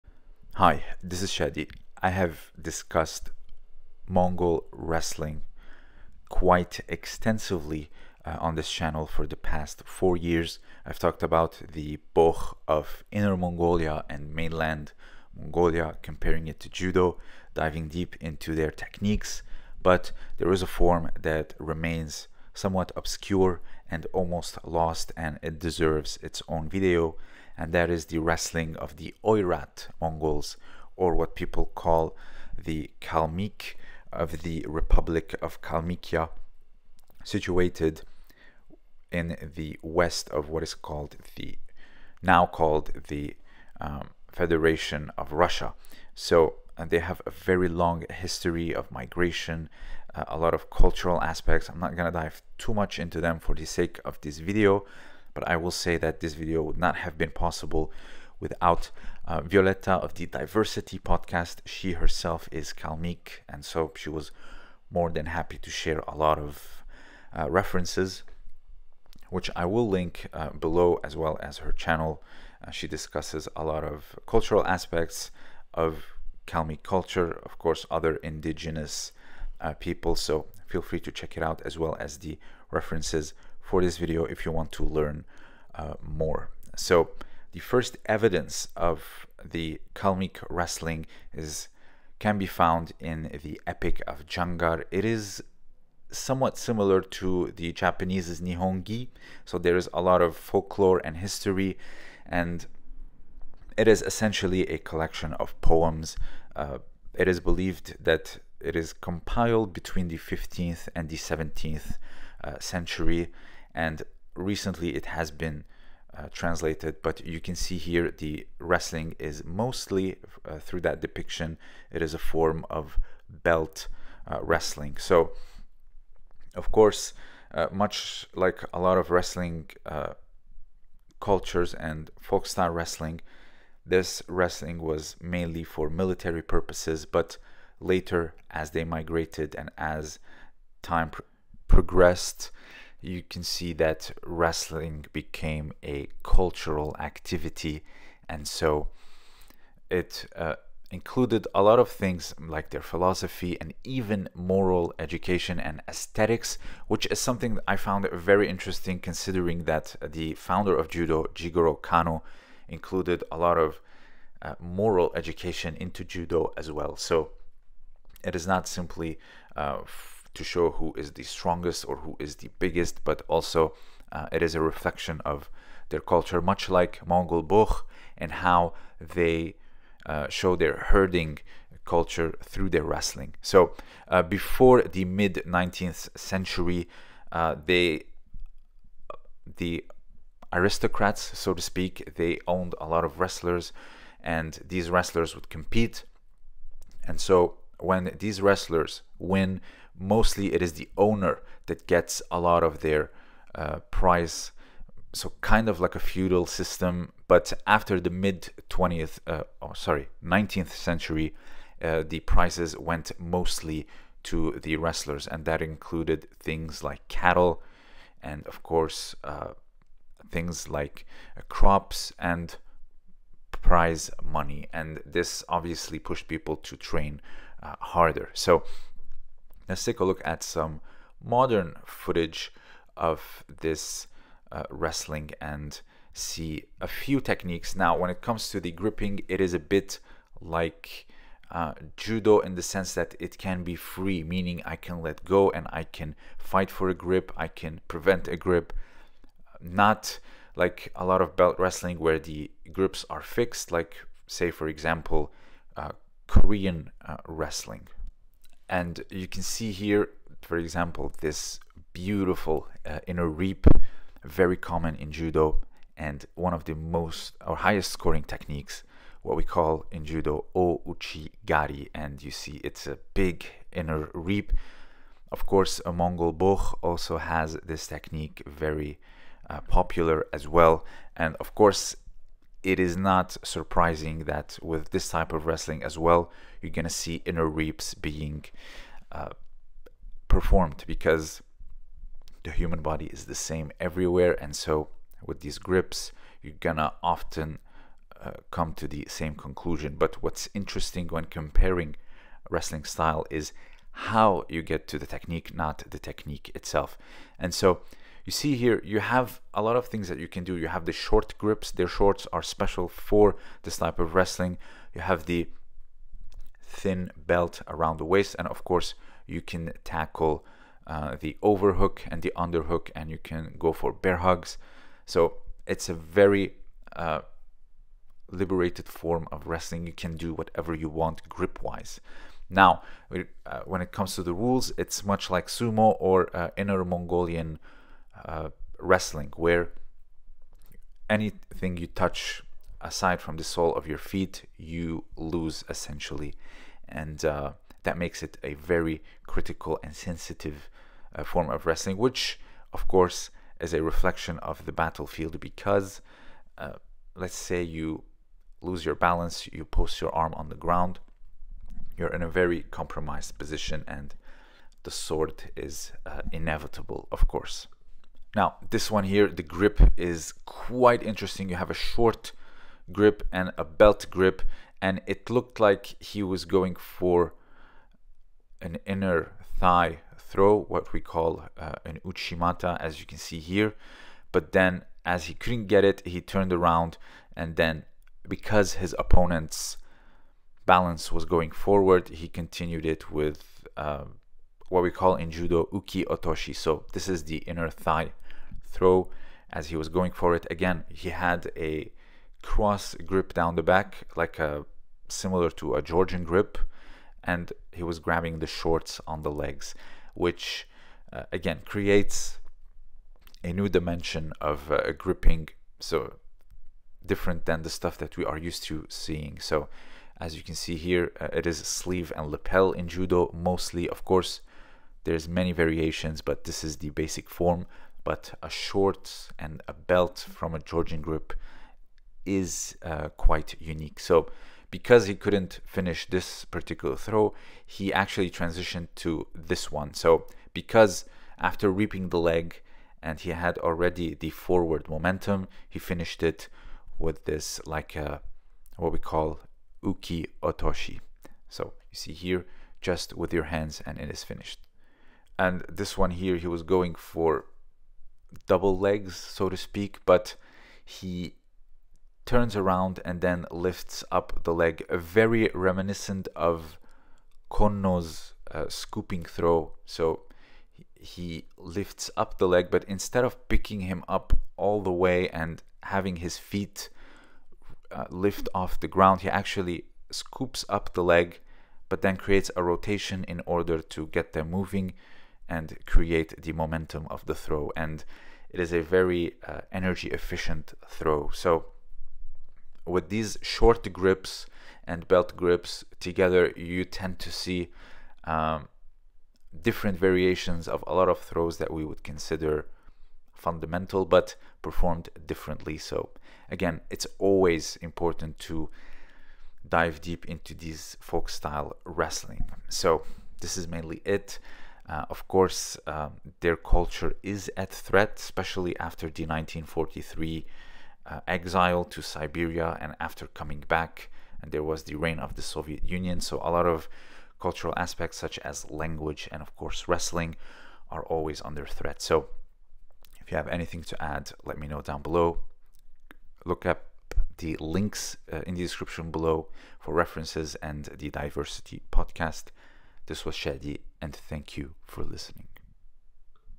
This form of Mongolian wrestling is almost lost
This video discusses the wrestling of the Oirad Mongols, also known as the Kalmyk.